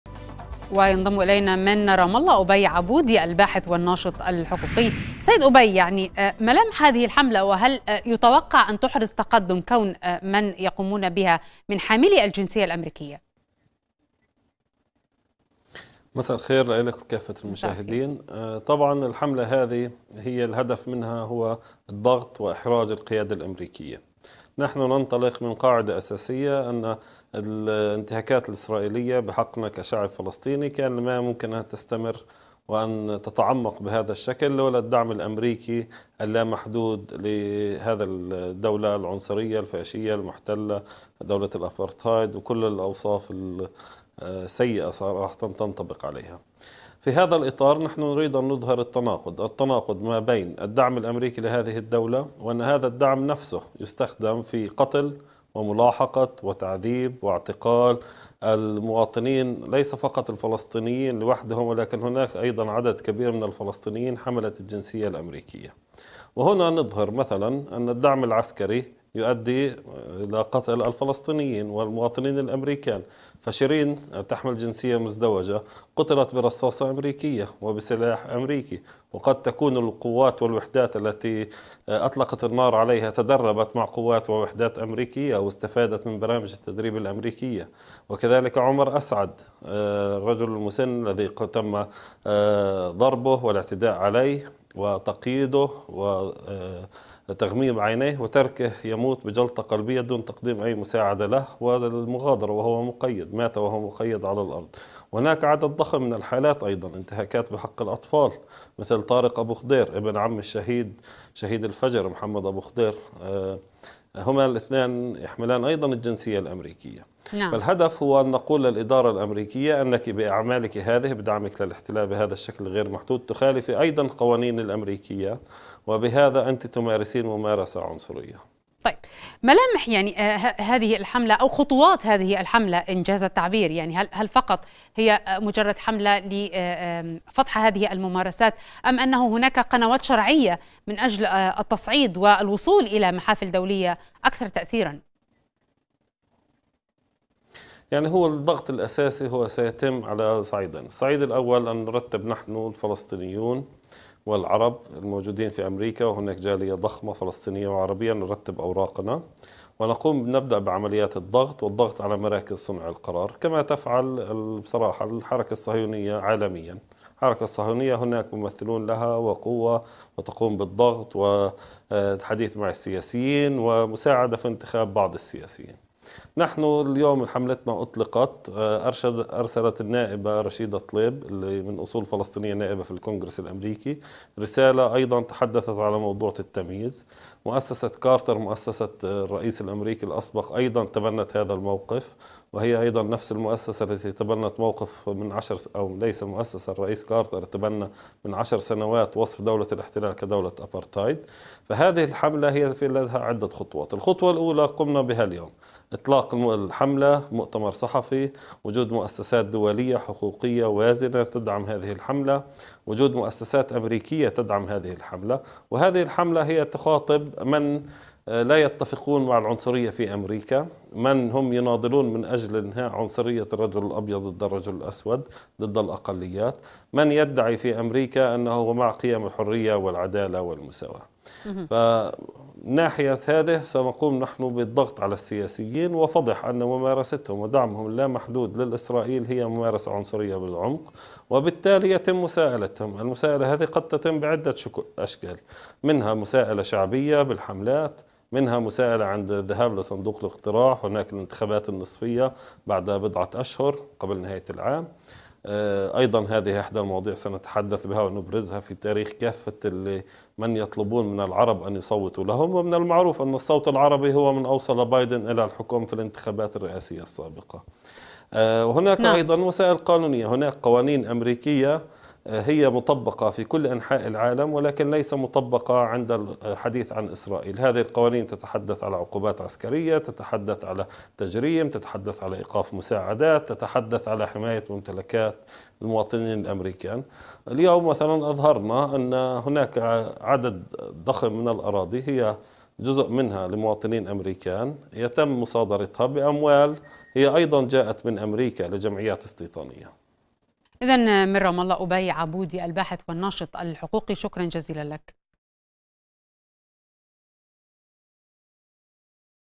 حملة حقوقية لحماية الفلسطينيين الأمريكيين من انتهاكات سلطات الاحتلال.. ما أهدافها؟
أطلق عدد من المواطنين الفلسطينيين من حاملي الجنسية_الامريكية، حملة لمطالبة الرئيس بايدن بحماية الفلسطينيين الامريكيين من انتهاك سلطات الاحتلال لحقوقهم. واستعرضت الحملة في وثيقة لها الانتهاكات التي يتعرض لها الفلسطينيون من حملة الجنسية الأمريكية، ومنها إعدام الشهيدة شيرين_أبو_ عاقلة وثلاثة اخرين برصاص قوات الاحتلال إضافة إلى عمليات التعذيب التي تعرض لها عدد آخر وتقييد حرية الحركة ومصادرة أراضيهم. كما طالبت الحملة الرئيس بايدن باحترام مبدأ المساواة بين المواطنين وفرض عقوبات على إسرائيل لانتهاكها حقوق الفلسطينيين... المزيد من التفاصيل يتحدث من رام الله أبي عابودي الباحث والناشط الحقوقي، مع سينار سعيد، عبر الفقرة الإخبارية.